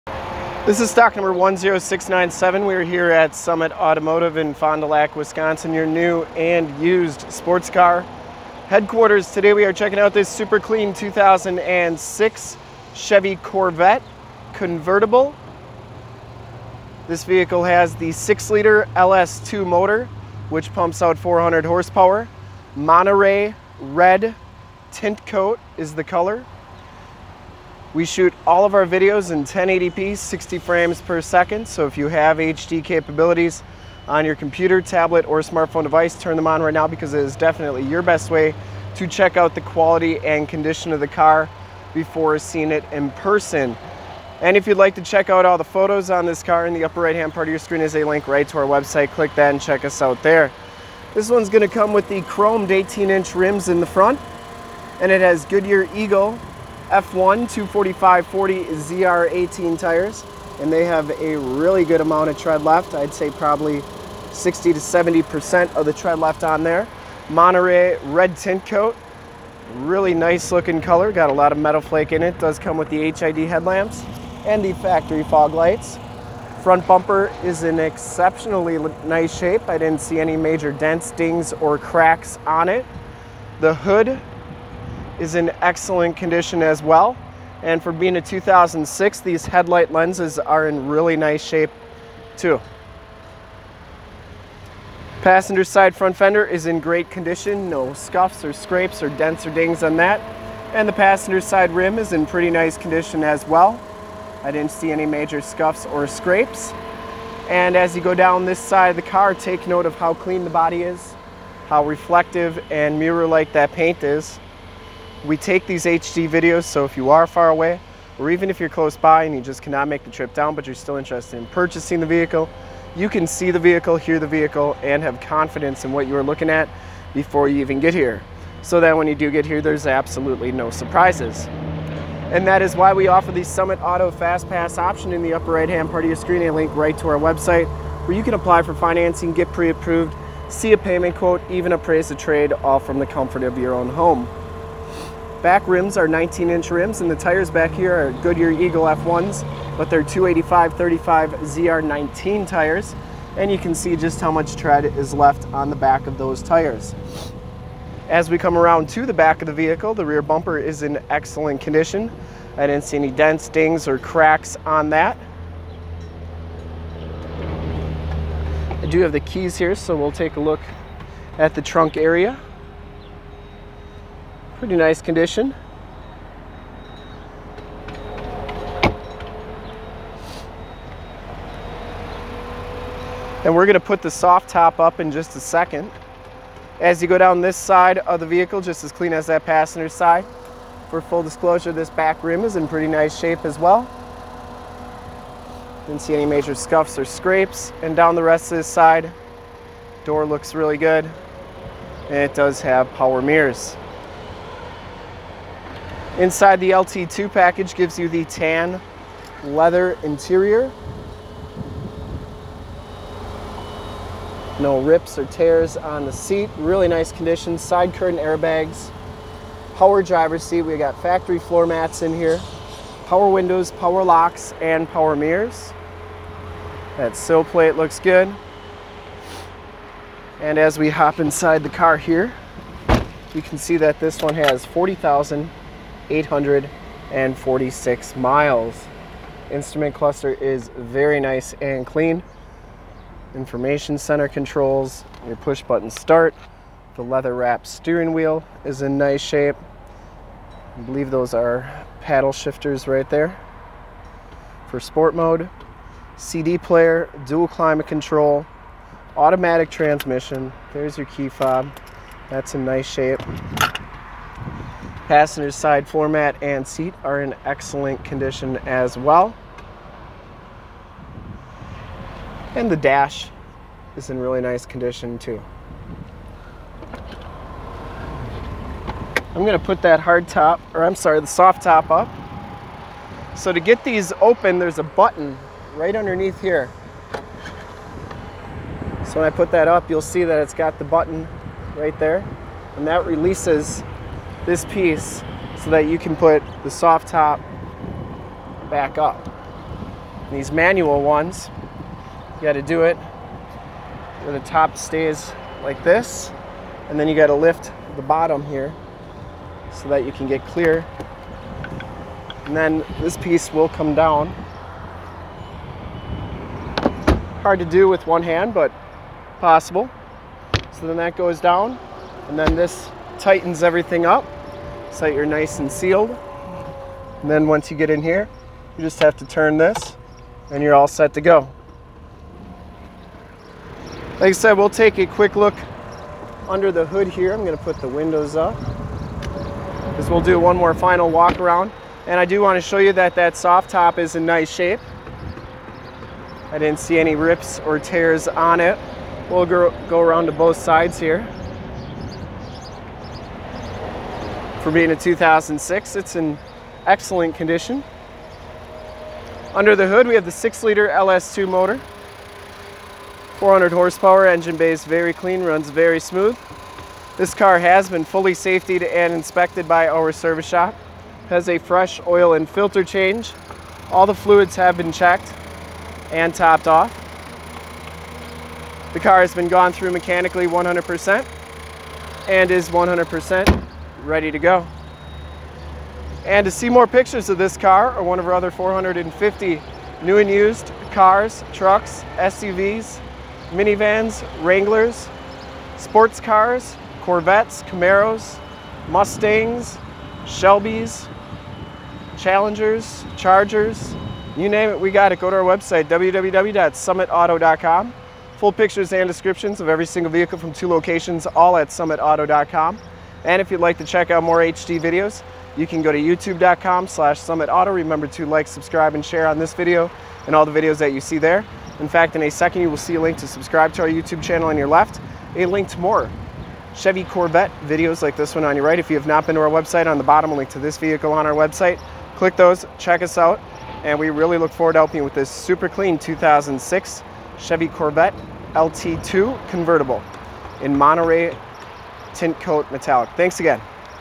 2006 CHEVROLET CORVETTE CONVERTIBLE LT2 MONTEREY RED WALK AROUND REVIEW 10697 SOLD! SUMMITAUTO.com
This 2006 CHEVROLET CORVETTE CONVERTIBLE LT2 IN MONTEREY RED METALLIC TINTCOAT is the vehicle we did walk around review of today.

Thank you for checking out this video of this 2006 CHEVROLET CORVETTE CONVERTIBLE LT2 IN MONTEREY RED METALLIC TINTCOAT

STOCK: 10697
PRICE: $22,999
MILES: 40,814
MAKE: CHEVROLET
MODEL: CORVETTE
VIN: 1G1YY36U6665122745
LOCATION: FOND DU LAC OSHKOSH WISCONSIN, 54937 TRUCKS ON 41

CLEAN TITLE HISTORY! TEXAS CAR! 6.0 Liter V8 Engine, 400 Horsepower, Two Door Convertible, LT2 Package 2LT LT, Automatic Transmission Center Console Shifter, Rear Wheel Drive RWD 4x2 Two Wheel Drive, Dual Rear Exhaust, Dual Power Seats, Tan Leather Seats, Bucket Seats, Power Mirrors, Stabilitrak Traction Control, Goodyear Eagle F1 P285/35 ZR19 Rear Tires, P245/40 ZR18 Front Tires, Factory Chromed Alloy Rims Premium Wheels, Four Wheel Disc Brakes, Fog Lights, HID High Intensity Discharge Headlights, Chrome Tipped Exhaust, AM / FM Radio Tuner, CD Player, Keyless Entry System, Push Button Start, Rear Window Defroster, Driver and Passenger Front Air Bags, Side Curtain Air Bags SRS Safety Restraint System, Mileage Display, Outside Temperature Display, Dual Multi-Zone Climate Control , Air Conditioning AC, Cruise Control, Power Locks, Power Windows, Tilt Steering Wheel, Automatic Headlights Autolamp, Monteray Red Metallic Maroon, CLEAN AUTOCHECK! TEXAS CAR! Very very clean inside and out! This is one of the sharpest 2006 Chevrolet Corvette cars we have ever had on our lot! Make your move before this super clean ride is gone!

STOCK: 10697
PRICE: $22,999
MILES: 40,814
MAKE: CHEVROLET
MODEL: CORVETTE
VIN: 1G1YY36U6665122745